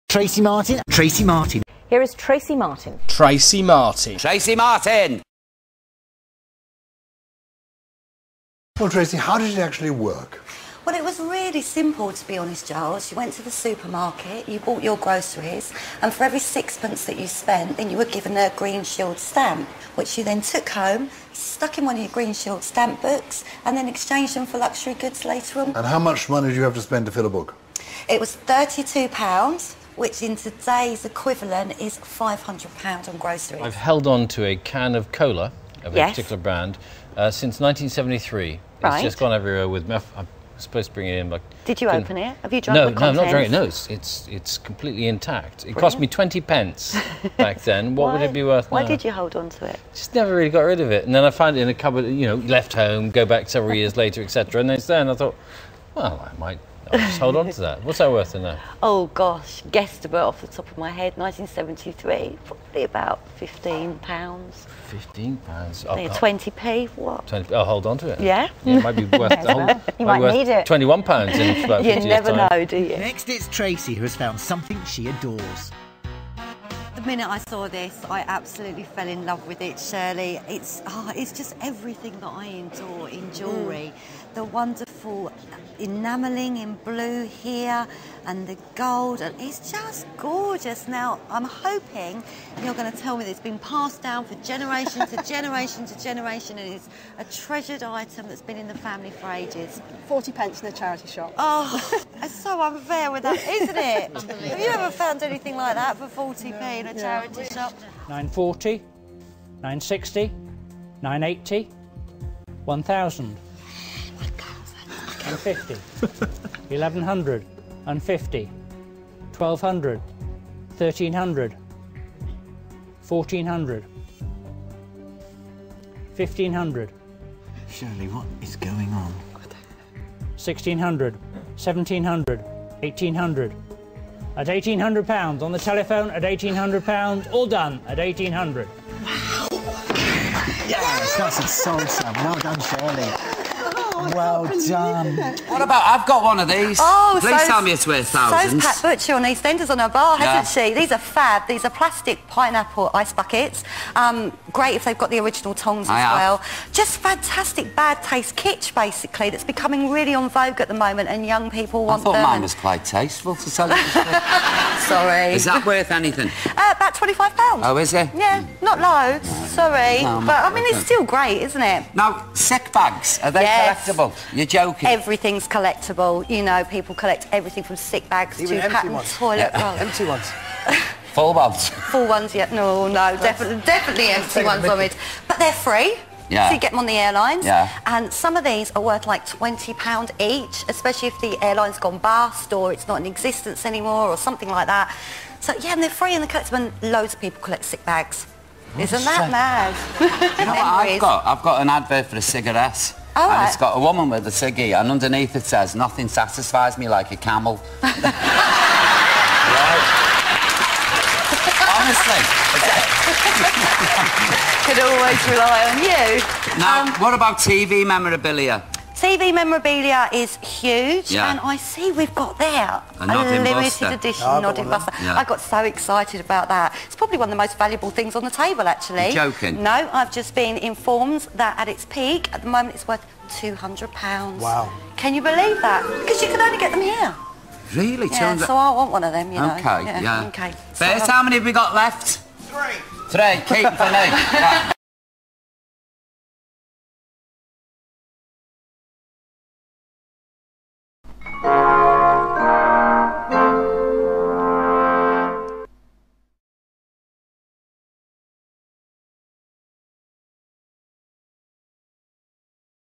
Tracy Martin Showreel
A showreel featuring some of the television appearances of Collectables Expert Tracy Martin